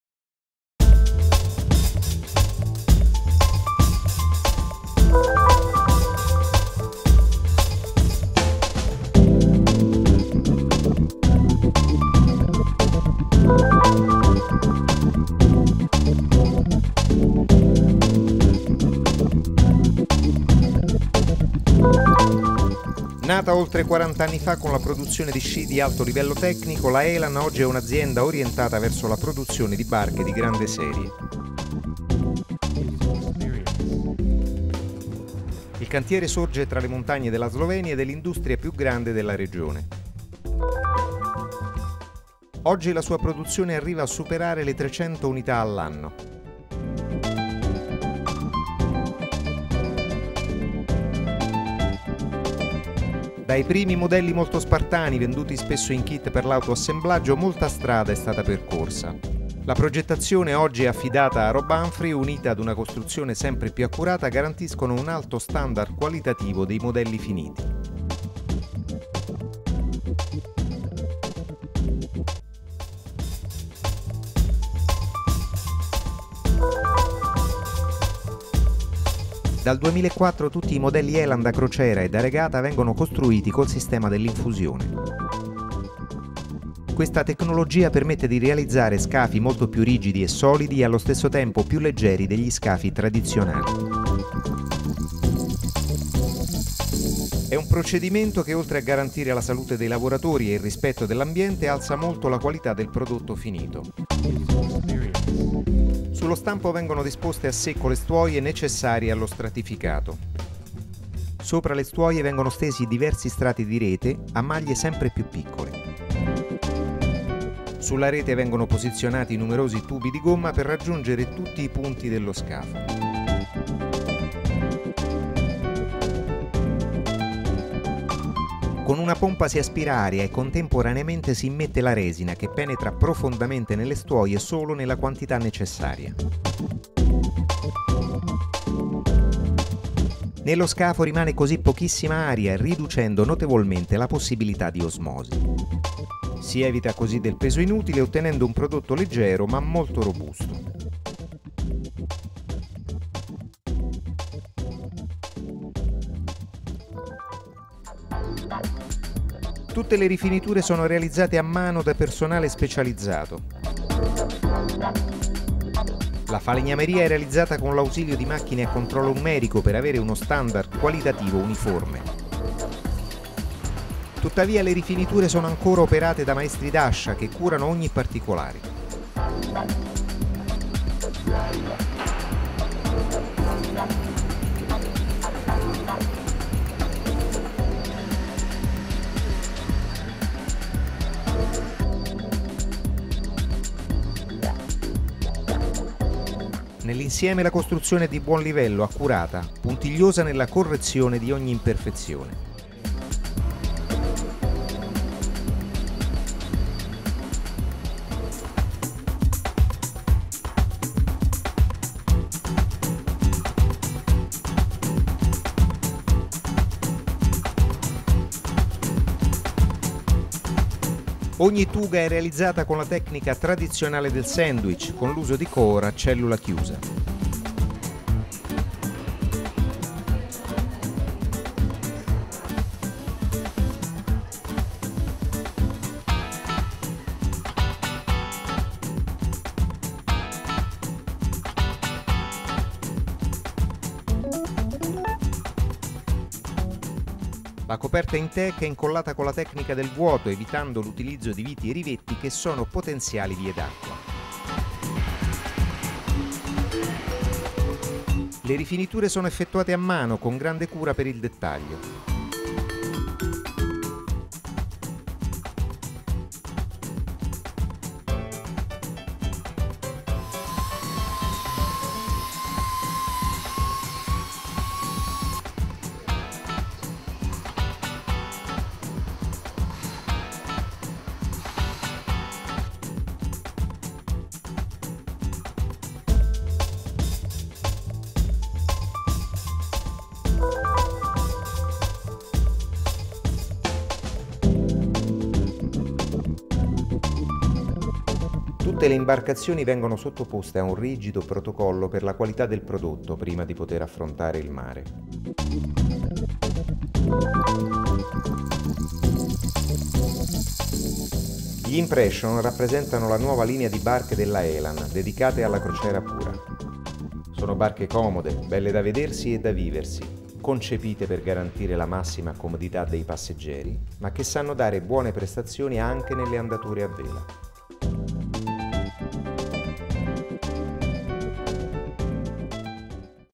Cantiere Elan
Video realizzato nel 2006 - Nato oltre quarant'anni fa tra tra le montagne della Slovenia, il cantiere Elan è stata dapprima un'azienda produttrice di sci.
Oggi è uno dei cantieri più importanti per il mercato del Mediterraneo. E' un cantiere molto dinamico i cui disegni sono firmati da Rob Humphreys